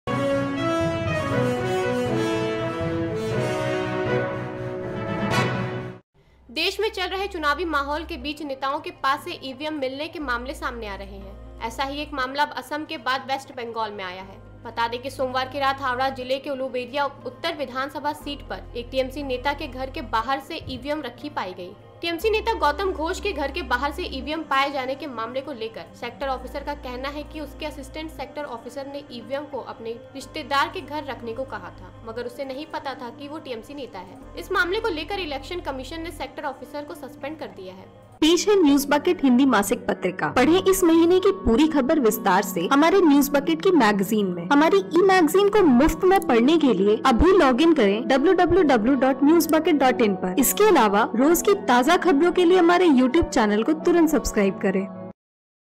West Bengal में TMC नेता के घर के बाहर मिली EVM, Sector Officer Suspend | EVM Scandal | News Bucket
देश में चल रहे चुनावी माहौल के बीच नेताओं के पास से EVM मिलने के मामले सामने आ रहे हैं। ऐसा ही एक मामला अब असम के बाद वेस्ट बंगाल में आया है। बता दें कि सोमवार की रात हावड़ा जिले की उलुबेरिया उत्तर विधानसभा सीट पर एक टीएमसी नेता के घर के बाहर से ईवीएम राखी पायी गयी है। Watch the complete Video For More Details!

Gadgets We Use:

Green Chroma